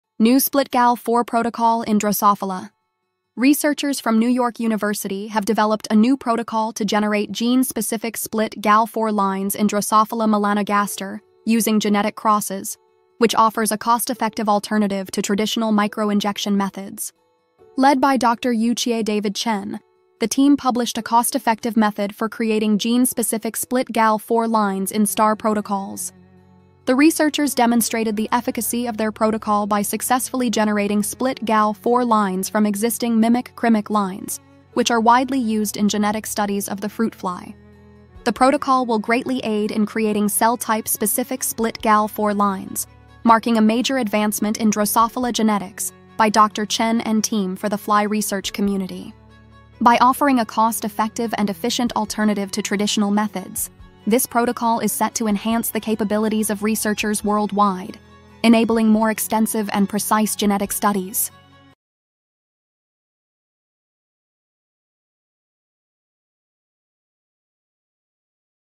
New Split GAL4 Protocol in Drosophila
New Cost-Effective Gene Editing in Drosophila! 🪰

Researchers at New York University have developed a cheaper, simpler method for creating gene-specific split-GAL4 lines in fruit flies. 🚀 No more expensive microinjections – this new protocol uses genetic crosses instead! 💡

Dr. Yu-Chieh David Chen's team highlights the method's accessibility and efficiency, making it ideal for studies in neurobiology, genetics, and developmental biology. 🌱